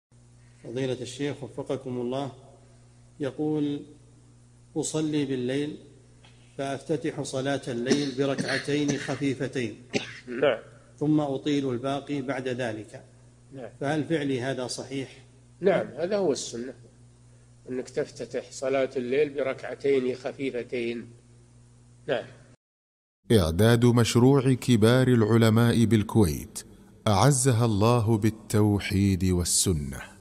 [516 -582] ما حكم افتتاح صلاة الليل بركعتين خفيفتين ثم الإطالة في الباقي؟ - الشيخ صالح الفوزان
-"السؤال: أصلي بالليل فأفتتح صلاة الليل بركعتين خفيفتين ثم أطيل الباقي بعد ذلك فهل فعلي هذا صحيح؟
الجَوَابُ: نعم هذا هو السُّنّة أنك تفتتح صلاة الليل بركعتين خفيفتين.
للاشتراك في قناة الشيخ صالح الفوزان
سؤال وجواب من شرح صفة الصلاة ( 582 ) مقطع

للاستفسار والإقتراحات والدعم
رئيس الفريق
لمشروع كبار العلماء